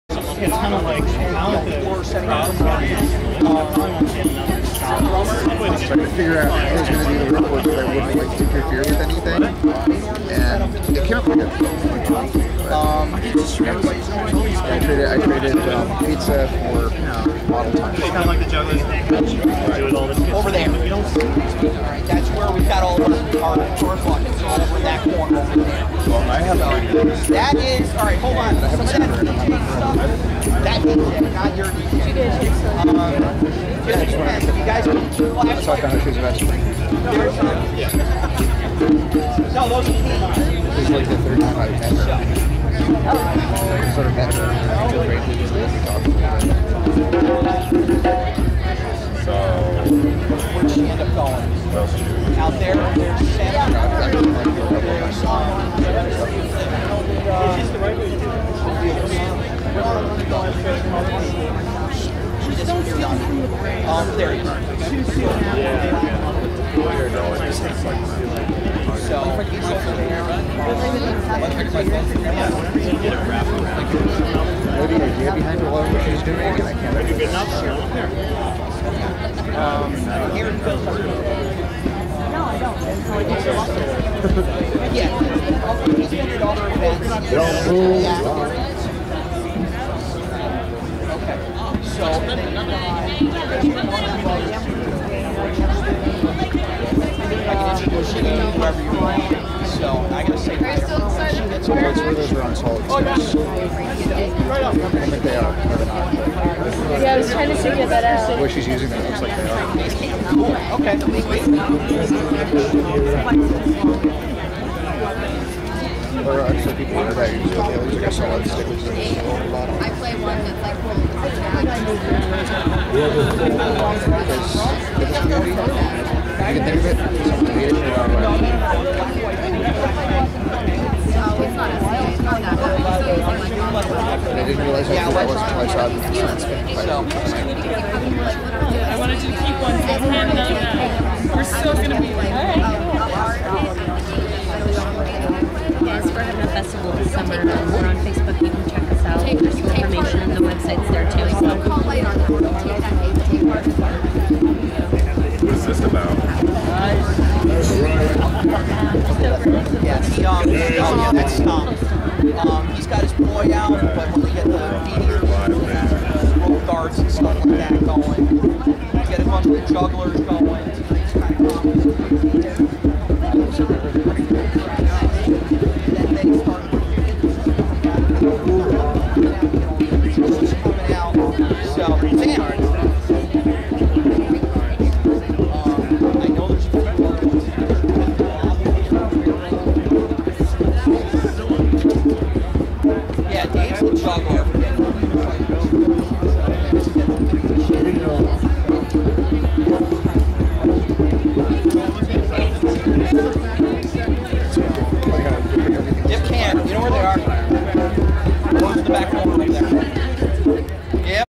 firespinning at spinjam
Among the spinners is Stomp. Despite being by accident, I like how the focus is lost for a short period of time.